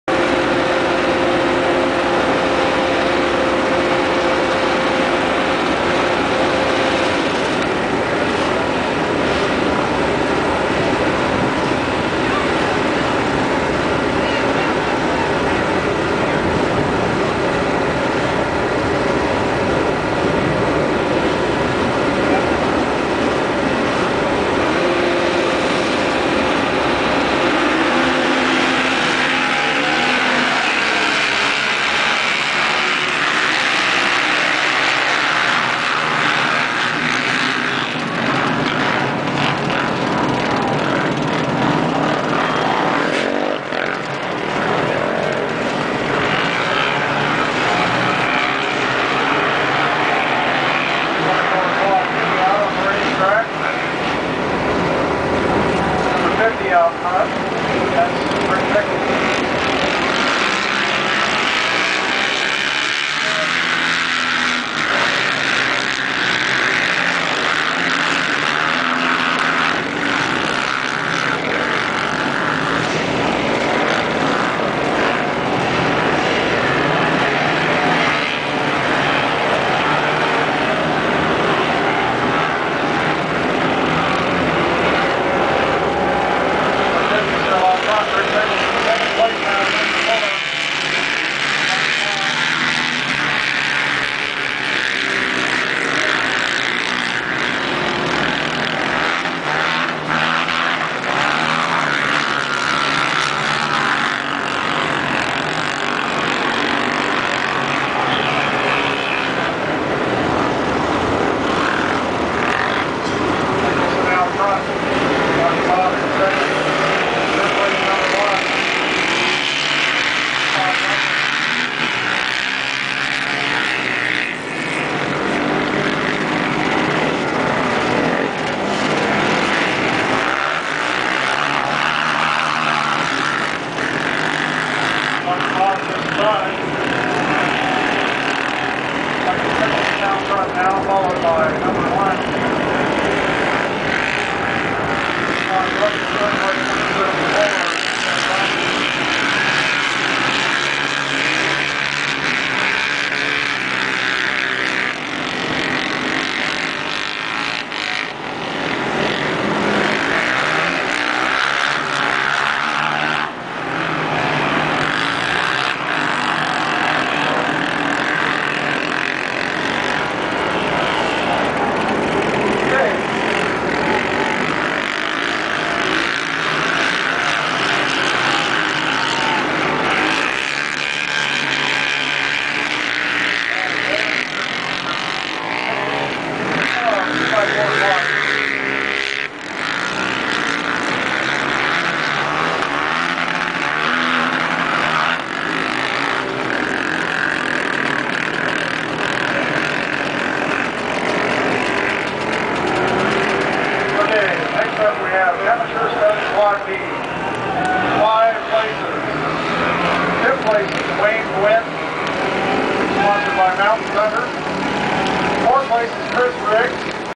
final Ice race of 2011 2x4 B
last race of the season for harrys roamers mc snow, slush,mayhem in the 2x4 B class atv, quad